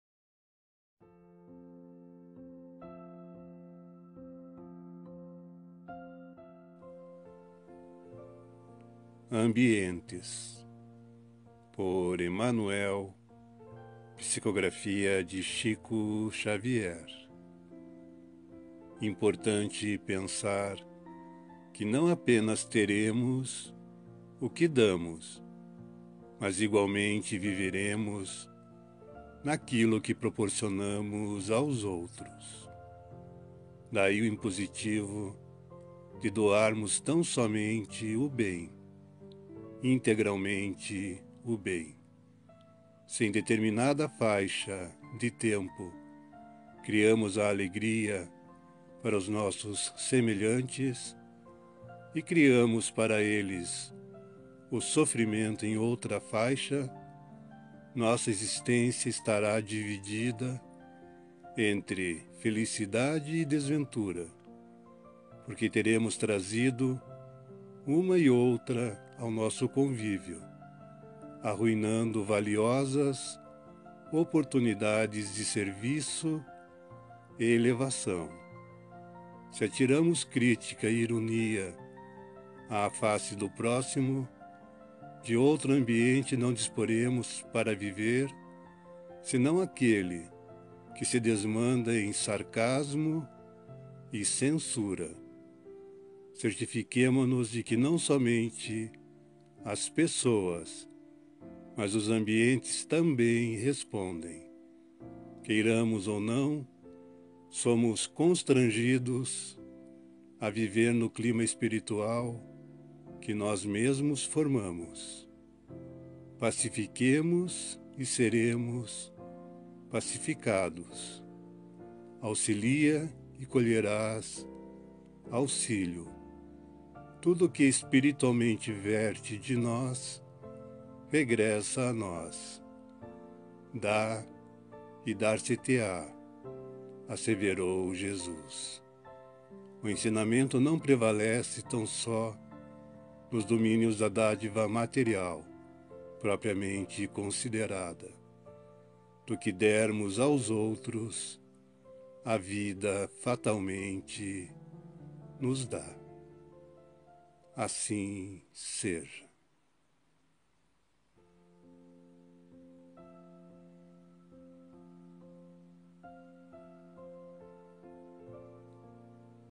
Ambientes! Emmanuel/Chico Xavier! SALVANDO VIDAS - ORAÇÕES E MENSAGENS PARA TODOS OS MOMENTOS!
Por: Emmanuel, Do livro: Alma e Coração, Médium: Francisco Cândido Xavier.
Ambientes
(Emmanuel/Chico Xavier)
"Importante pensar que não apenas teremos o que damos, mas igualmente viveremos naquilo que proporcionamos aos outros.
Daí o impositivo de doarmos tão somente o bem, integralmente o bem.
Se em determinada faixa de tempo criamos a alegria para os nossos semelhantes e criamos para eles o sofrimento em outra faixa, nossa existência estará dividida entre felicidade e desventura, porque teremos trazido uma e outra ao nosso convívio, arruinando valiosas oportunidades de serviço e elevação.
Se atiramos crítica e ironia à face do próximo, de outro ambiente não disporemos para viver senão aquele que se desmanda em sarcasmo e censura.
Certifiquemo-nos de que não somente as pessoas, mas os ambientes também respondem. Queiramos ou não, somos constrangidos a viver no clima espiritual que nós mesmos formamos. Pacifiquemos e seremos pacificados. Auxilia e colherás auxílio.
Tudo o que espiritualmente verte de nós, regressa a nós. “Dá e dar-se-te-á” — asseverou Jesus.
O ensinamento não prevalece tão só nos domínios da dádiva material propriamente considerada. Do que dermos aos outros, a vida fatalmente nos dá."